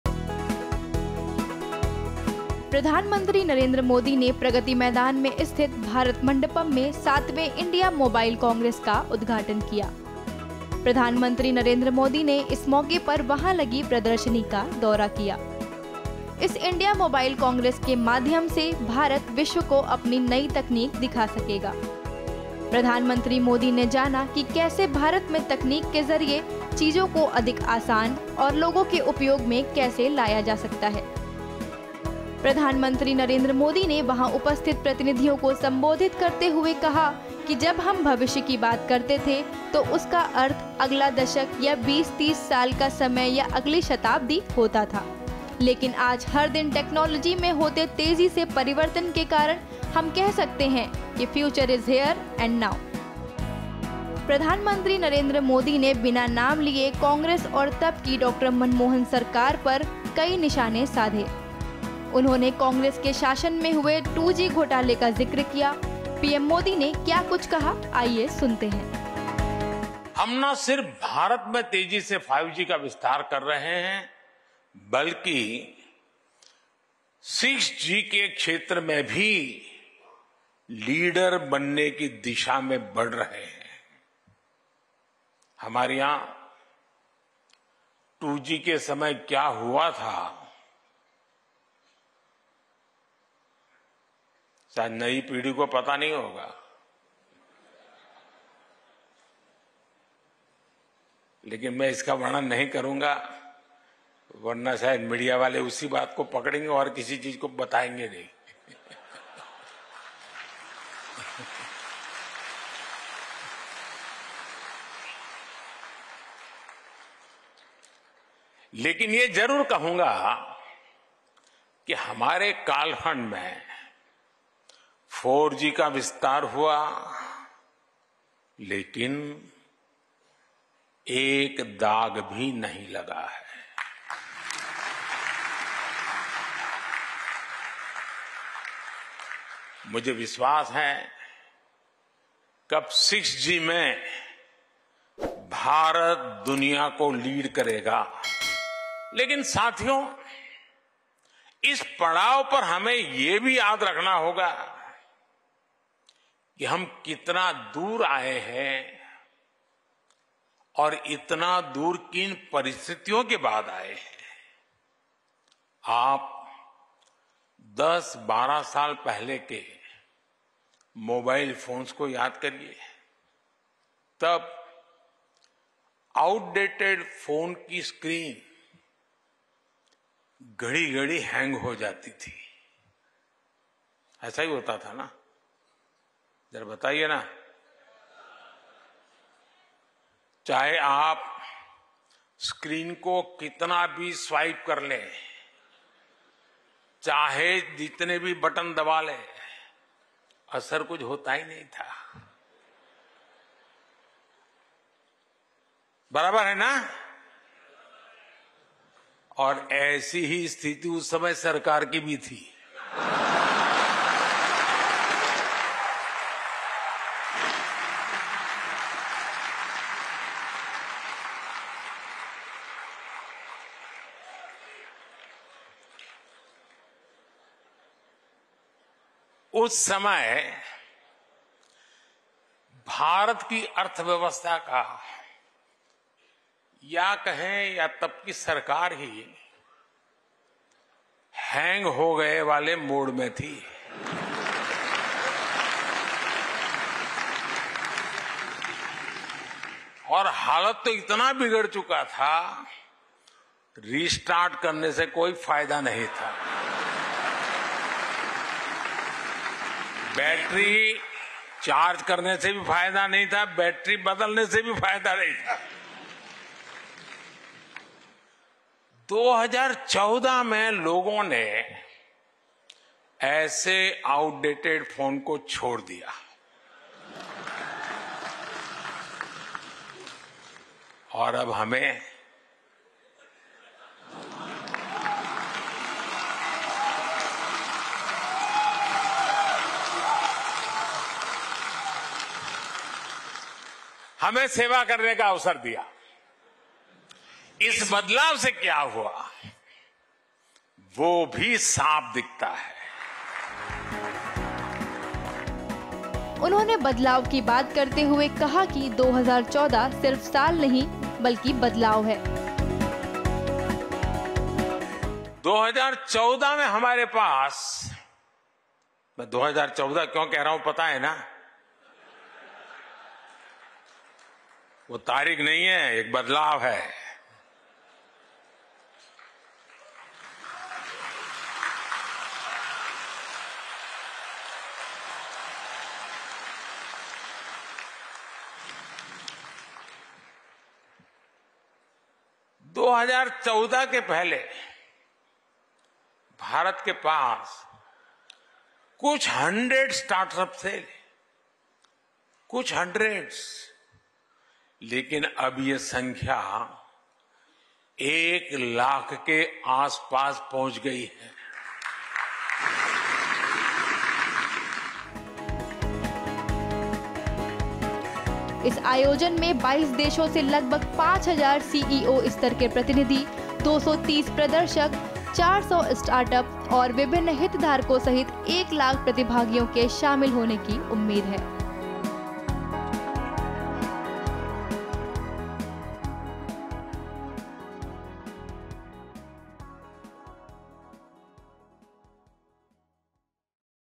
PM मोदी ने IMC का किया उद्घाटन, बिना नाम लिए कांग्रेस पर साधे कई निशाने
प्रधानमंत्री नरेंद्र मोदी (PM Narendra Modi) ने प्रगति मैदान (Pragati Maidan) में स्थित भारत मंडपम (Bharat Mandapam) में 7वें इंडिया मोबाइल कांग्रेस (India Mobile Congress) का उद्घाटन (Inauguration) किया। प्रधानमंत्री नरेंद्र मोदी ने इस मौके पर वहां लगी प्रदर्शनी का दौरा किया। इस कांग्रेस के माध्यम से भारत विश्व को अपनी नई तकनीक दिखा सके। प्रधानमंत्री मोदी ने जाना कि कैसे भारत में तकनीक के जरिए चीज़ों को अधिक आसान और लोगों के उपयोग में कैसे लाया जा सके वह जाना।